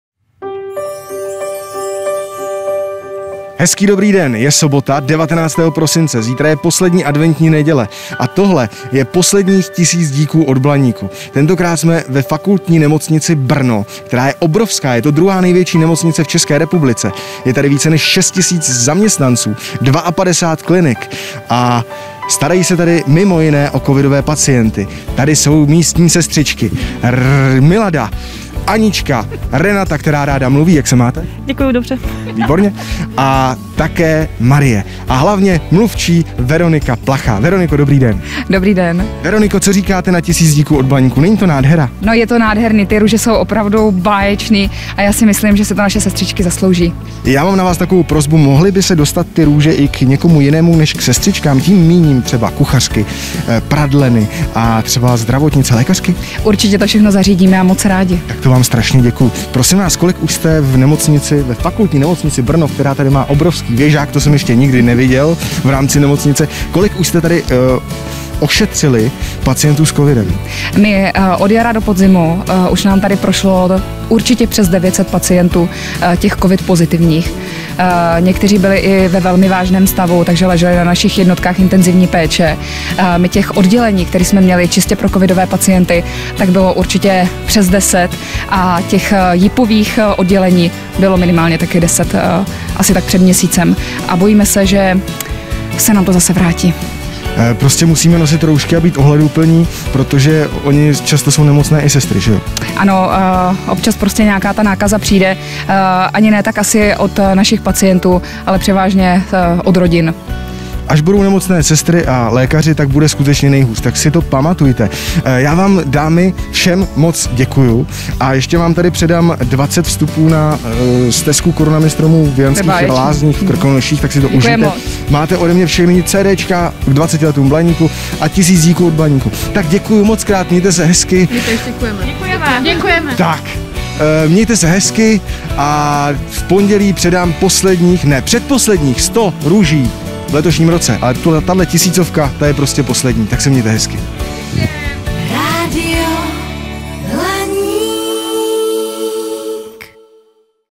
TISÍC DÍKŮ OD BLANÍKU! Fakultní nemocnice Brno (19 . 12. 2020)
Každou sobotu až do Vánoc přivezeme hrdinům v první linii 1000 růží!